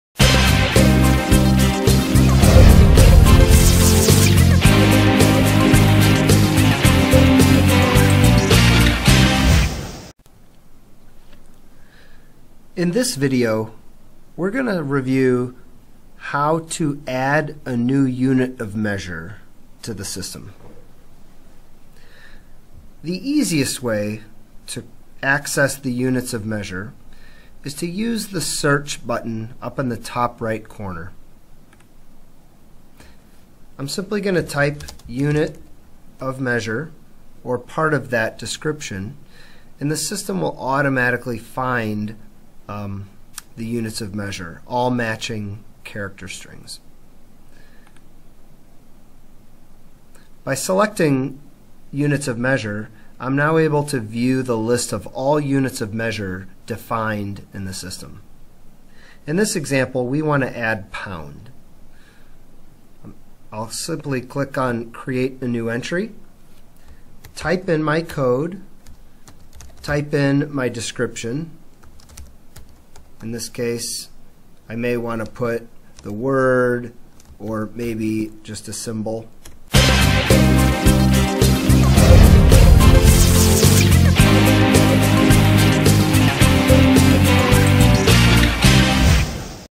How to Add a new Unit of Measure in Dynamics 365 for Financials - Project Madeira
How to add a new Unit of Measure in in Microsoft Dynamics 365 for Financials - Project Madeira.